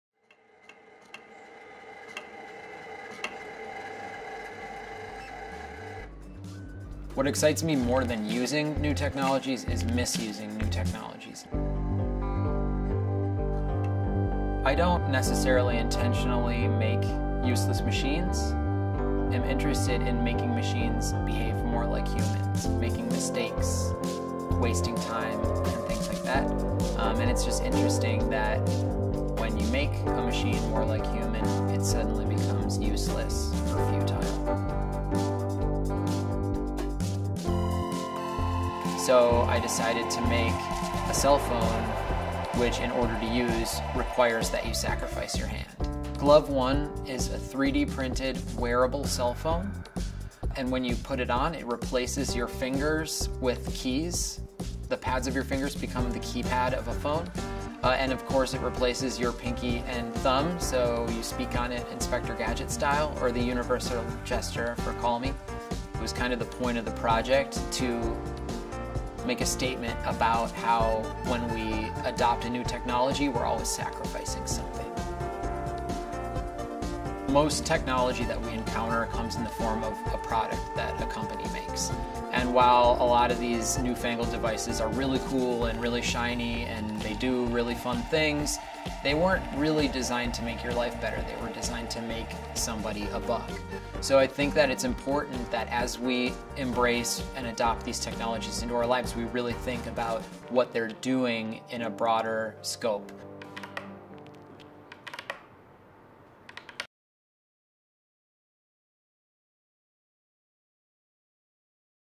Meet the Master of Useless Machines
Midwestern ingenuity doesn't always create valuable products.  Sometimes we only discover disturbing truths.  For example, "when you make a machine more human it suddenly becomes more useless".

Disturbing indeed.